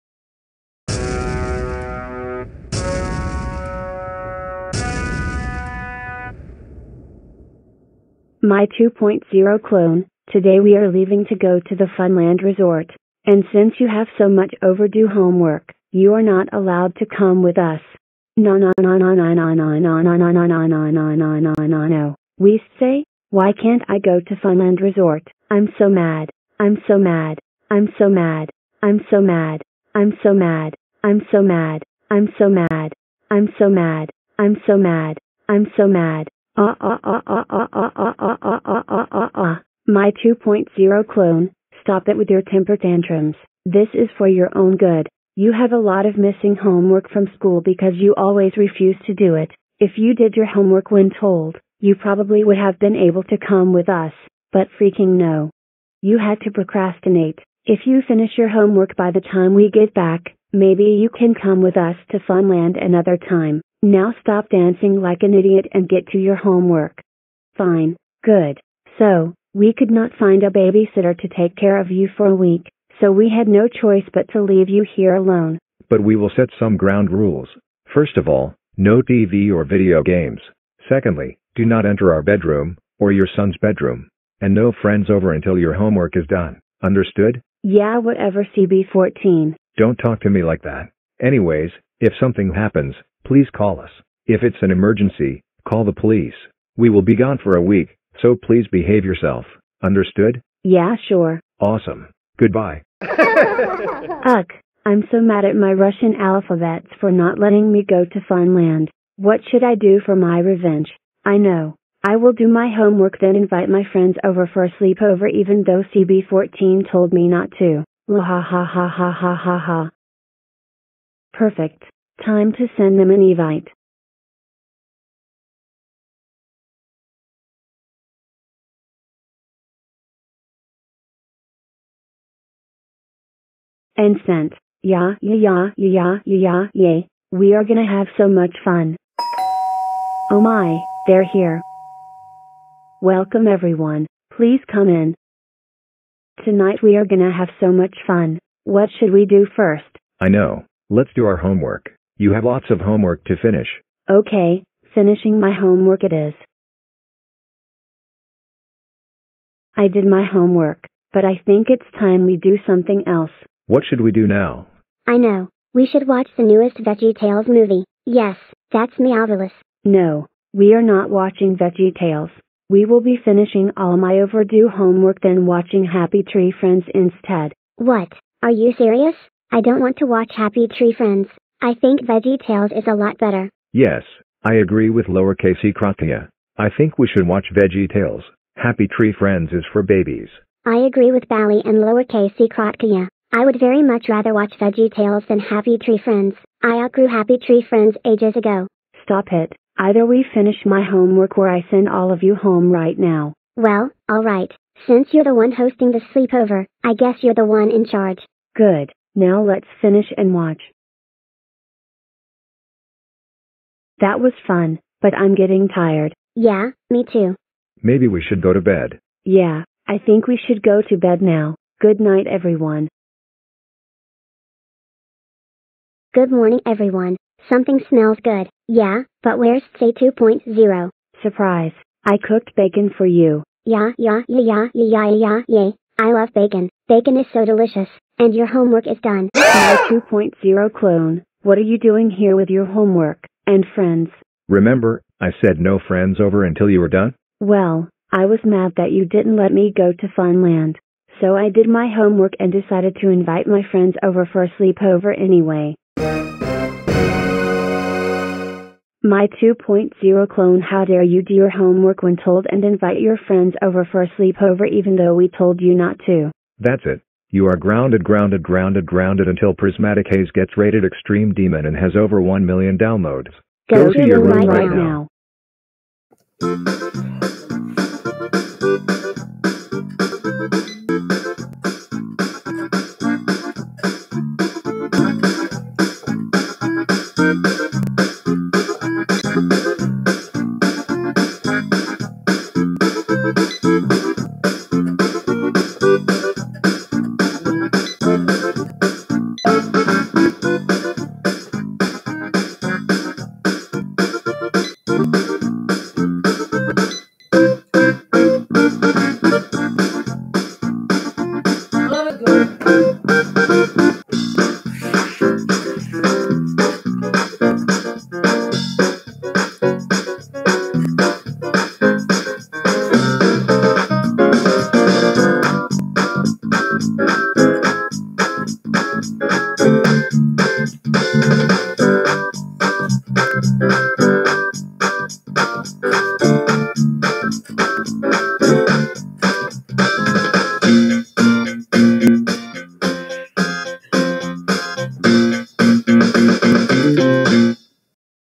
S4 EP7: Tseh 2 Does the Homework with Friends Over and Gets Grounded!
Tseh 2 does her homework this time with friends, what will she do.